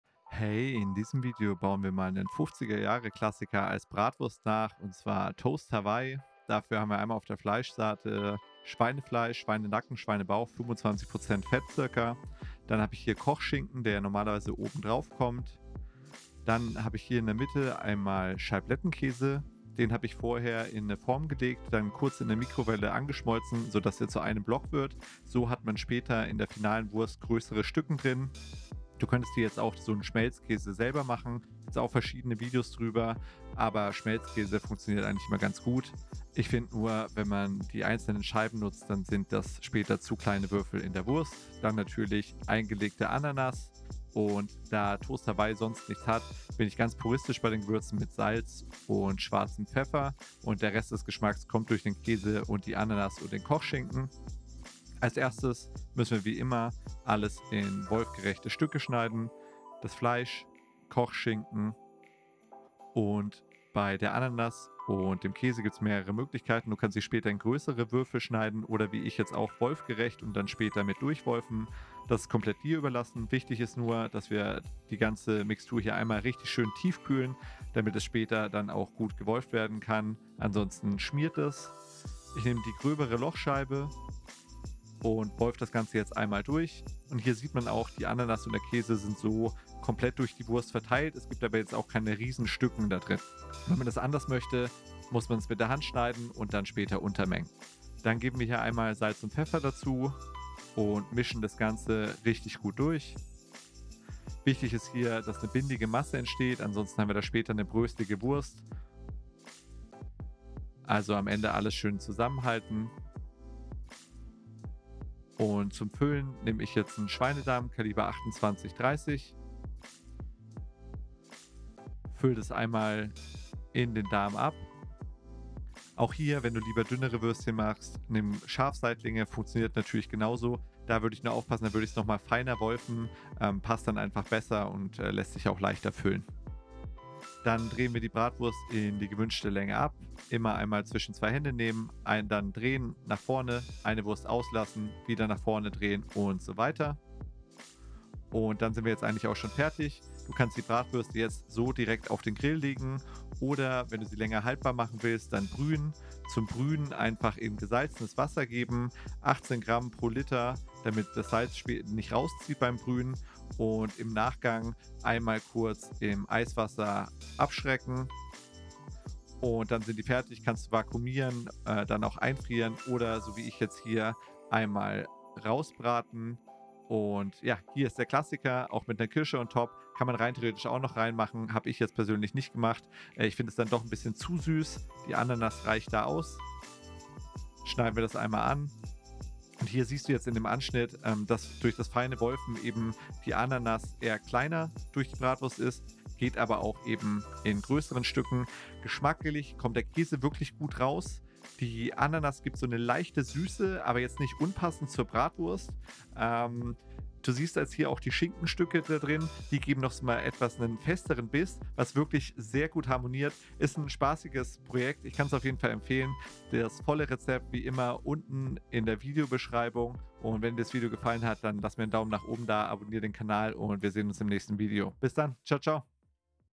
Hawaii Toast Bratwurst - 50er Jahre Klassiker als Wurst interpretiert
In diesem Video zeige ich dir, wie du Hawaii Toast Bratwurst selber machen kannst. Die Zutaten findest du in der Videobeschreibung und im Blogartikel.

🍽️ Das Rezept:
Zutaten pro 1 kg
30 % Schweinebauch (300 g)
25 % Schweinenacken (250 g)
15 % Kochschinken (150 g)
15 % Scheiblettenkäse (150g)
15 % Ananas aus der Dose (150 g)

Beim Käse ist es wichtig, dass er hitzestabil ist und wirklich cremig bleibt und sich nicht “trennt”. Dafür gibt es verschiedene Möglichkeiten, die einfachste ist Scheiblettenkäse bzw. “American Cheese”. Alternativ kann man jeden Käse mit z.B. Zitronensäure “untrennbar” machen. Ich werde dafür demnächst noch einen separaten Beitrag verfassen.

Gewürze pro kg
12 g Salz
2 g schwarzer Pfeffer
Optional Cocktailkirsche

Ich freue mich über jegliches Feedback, Anregungen und Anfragen. Schreib mir gerne eine E-Mail an: daniel(at)charcuteria.de